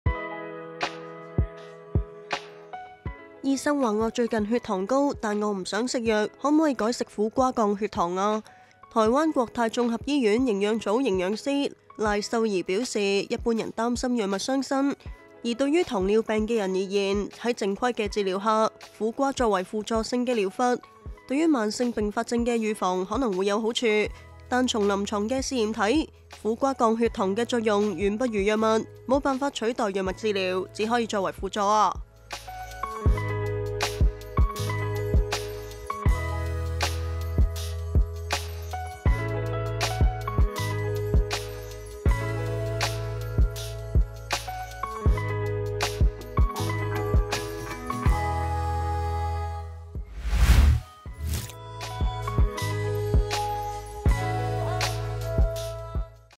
降血糖飲食｜苦瓜降葡萄糖吸收 功效媲美藥物？醫生這樣說...｜01教煮｜苦瓜｜糖尿病｜食療｜血糖
台灣國泰綜合醫院營養組營養師賴秀怡表示，一般人擔心藥物傷身，對於糖尿病人而言，在正規治療下，苦瓜作為輔助性的療法，對於慢性併發症的預防可能會有好處。